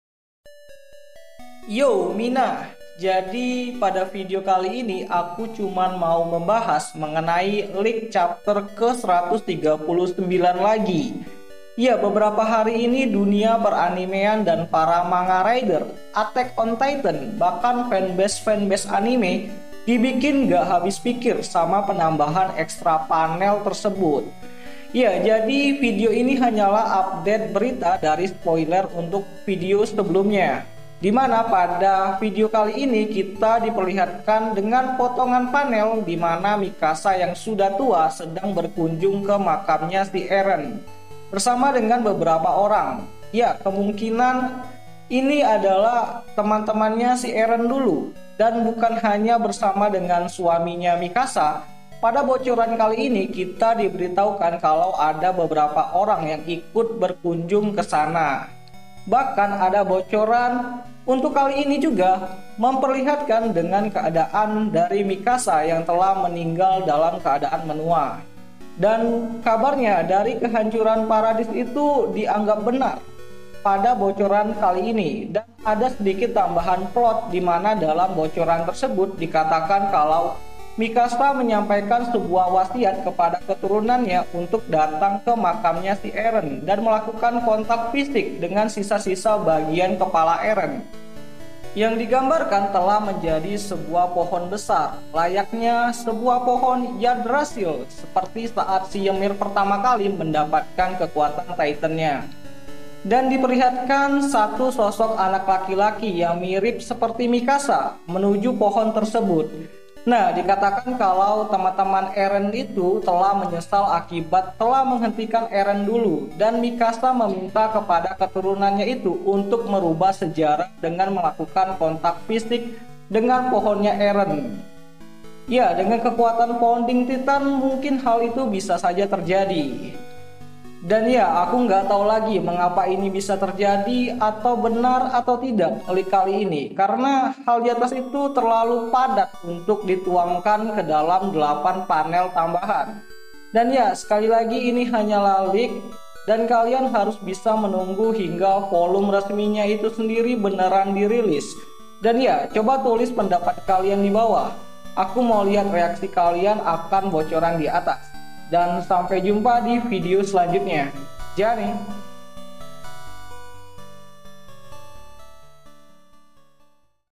Extra Panel Attack on Titan Chapter 139 Leak..
Keturunan terakhir Mikasa akan merubah sejarah ...

tambahan spoiler extra panel chapter 139 manga Attack on Titan..

Jadi spoiler untuk panel extra chapter 139 manga Attack on Titan akan menceritakan mengenai kisah cinta Mikasa dan Jean...

ngak mau. !!

MIKASA X JEAN..

EREN YEAGERS